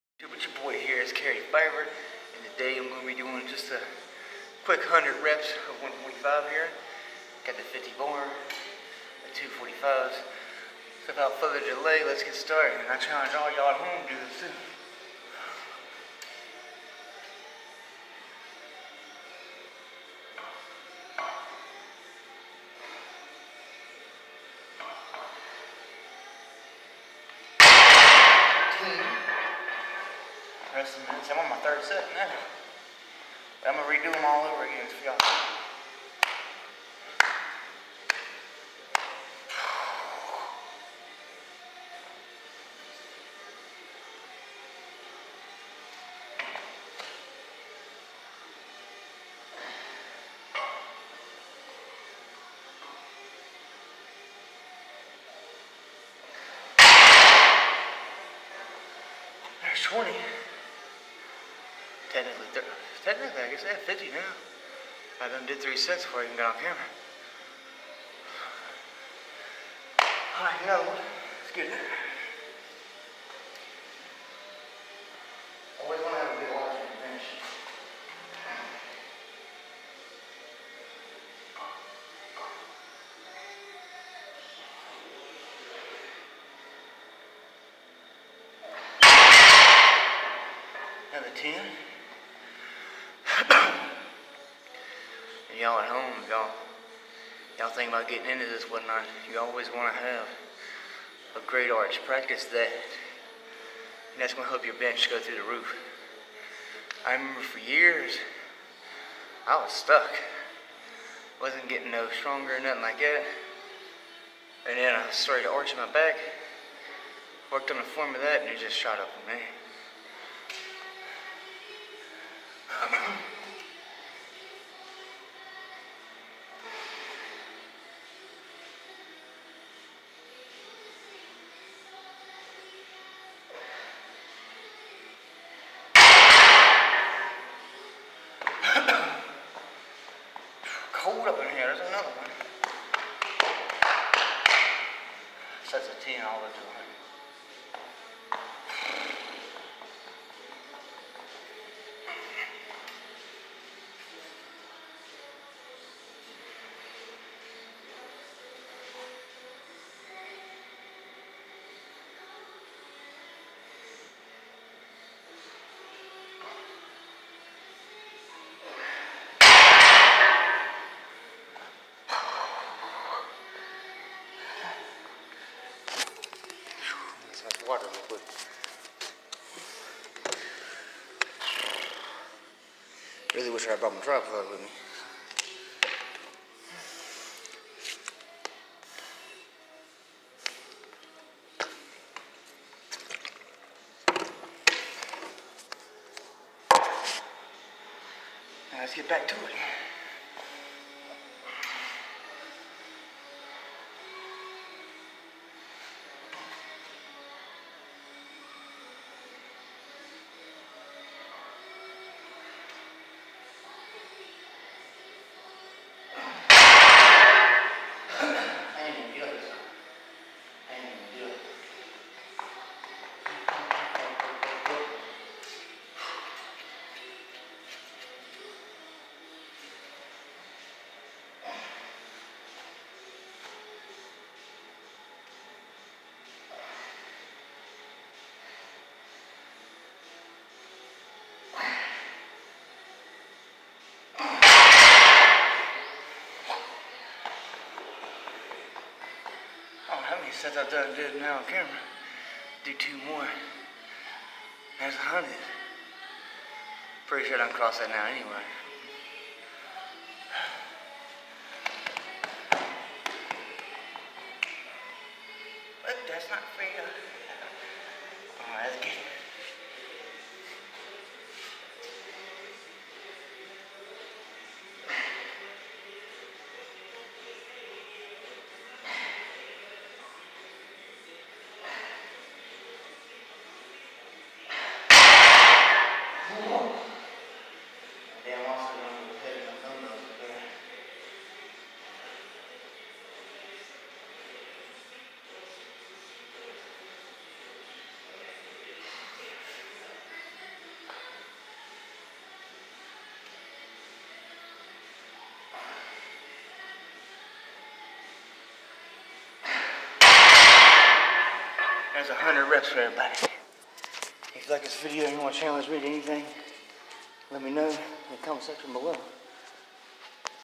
100 reps of 145 challenge
carrotyfiber does 100 reps of 145 on benchpress